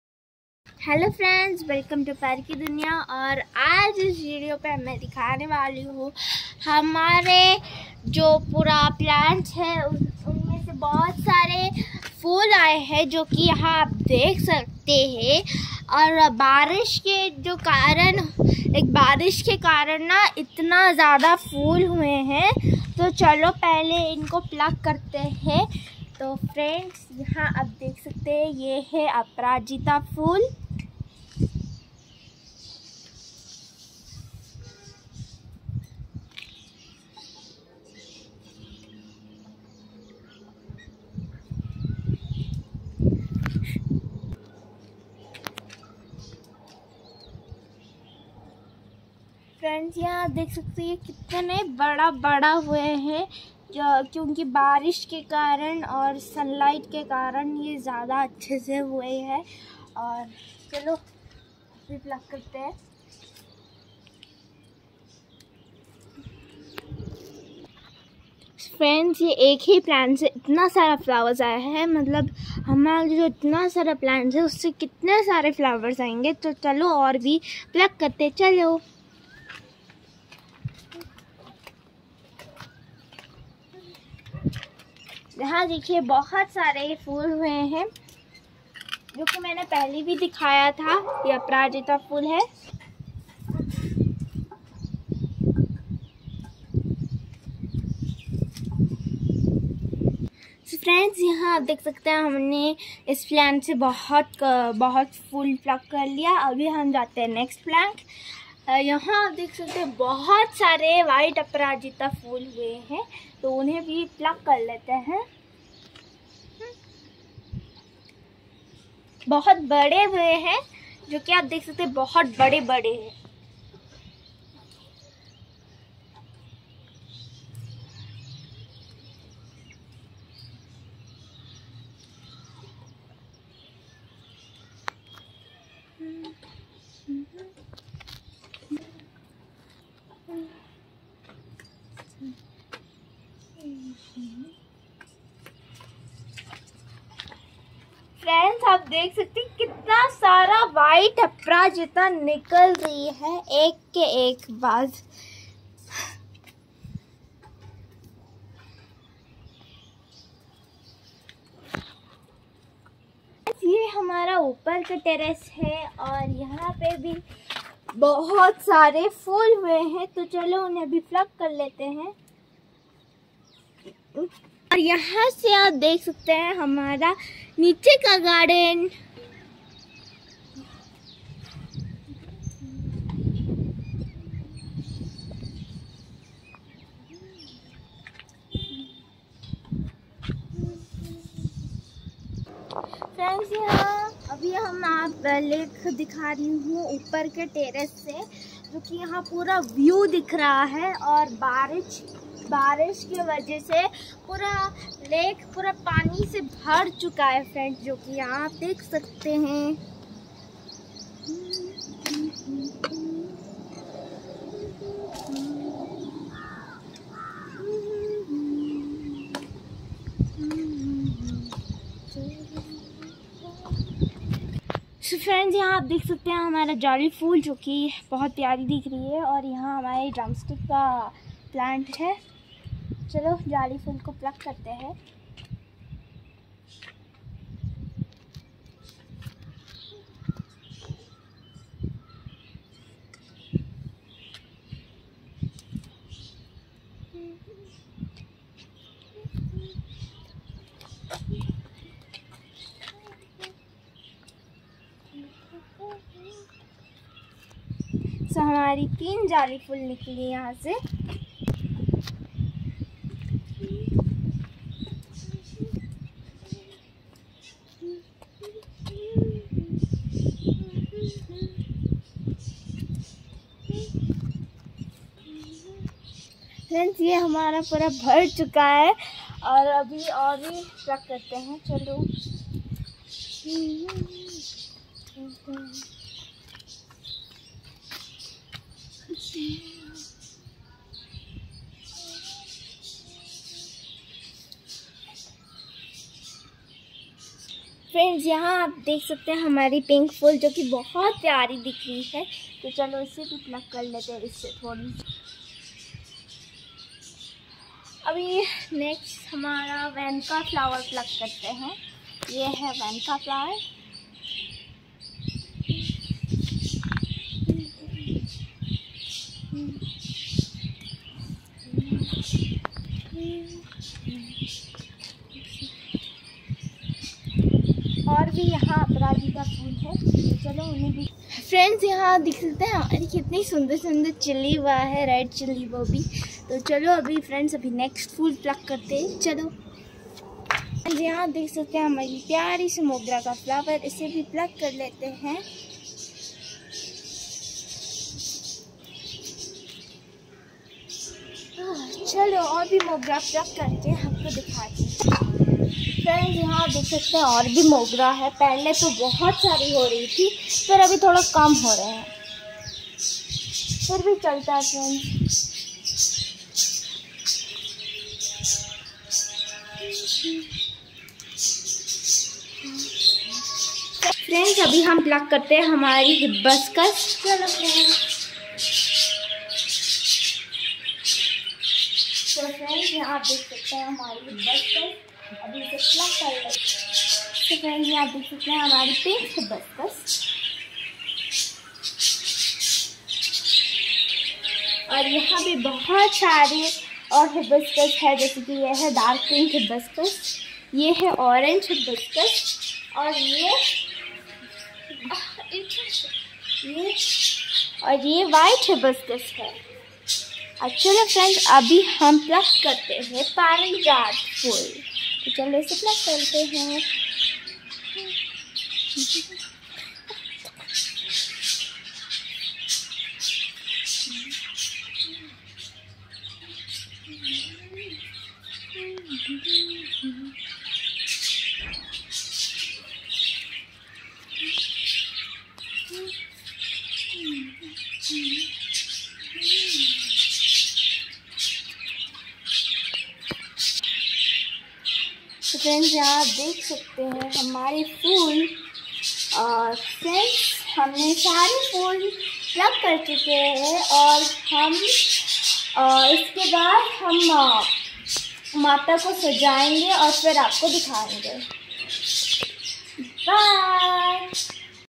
Attractive Flowers in my Terrace Garden, we offer to our GOD
Attractive Flowers in my Terrace Garden, we offer to our GOD. Pari Ki Duniya, It is a beautiful Channel for everyone, provides all kind of events, informations, ideas and places to visit.